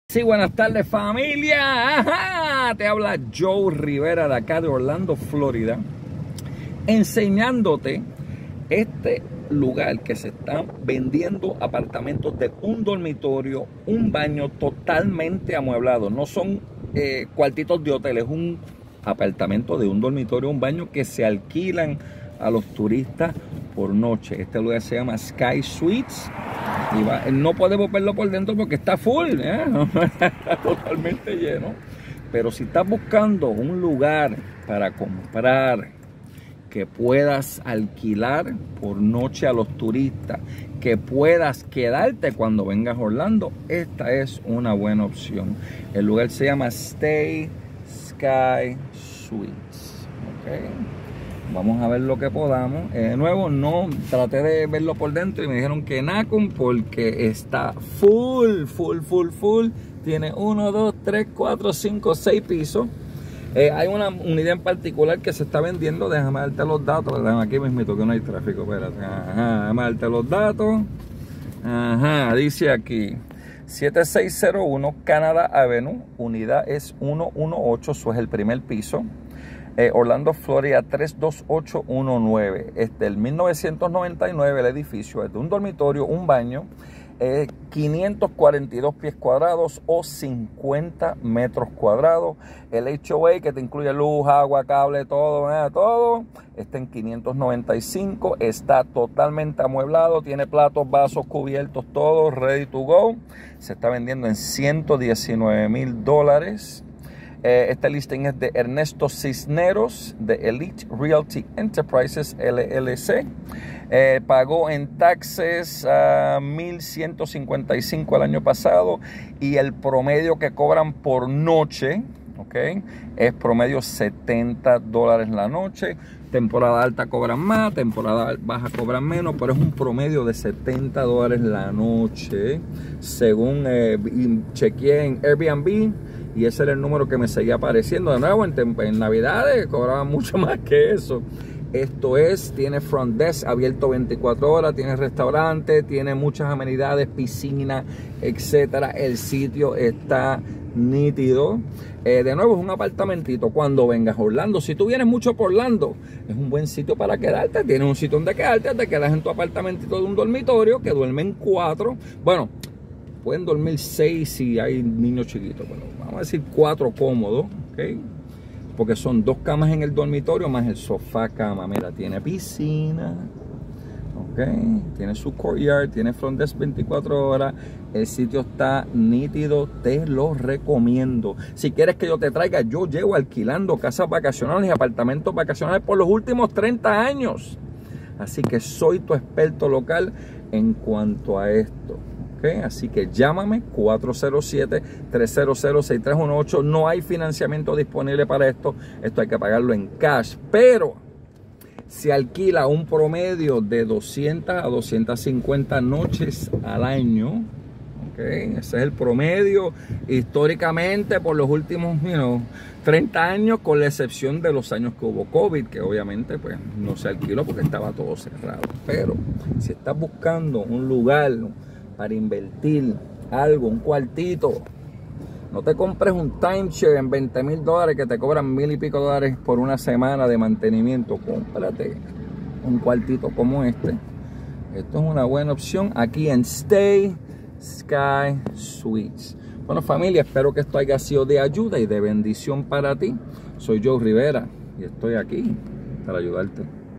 Se Vende Apartamento Amueblado de 1 Dormitorio 1 Baño en Orlando, Florida 32819
Joe Rivera
Property Outlet International

Este Video es Auspiciado por:
JJC Auto Repair
1198 East Carroll Street
Kissimmee, Florida 34744